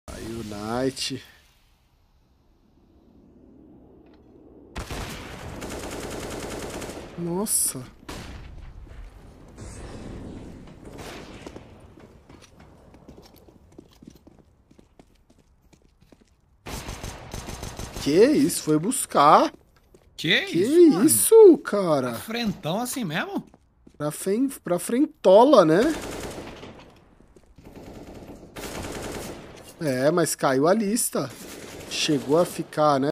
GAULES FICOU IMPRESSIONADO COM REMIX HUMILHANDO FALLEN!!!
VIDEO MOSTRA GAULES FICOU IMPRESSIONADO COM REMIX HUMILHANDO FALLEN!!!